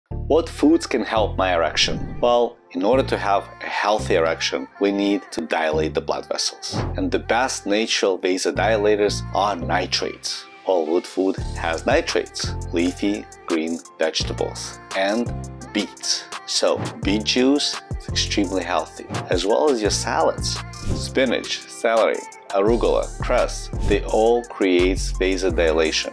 Erection boosting Food | Natural Viagra 2023
Cure Erectile Dysfunction without Pills in 3 weeks. Sign up for a free Consultation.

Erection boosting food | Natural Viagra 2022 for you.
What foods can help your Erection? What foods can help you if you have erectile dysfunction?
Dr. Arkady Lipnitsky will explain in this video which food you should eat to increase blood flow to your penis naturally.
Our clinic:
ReBalance NYC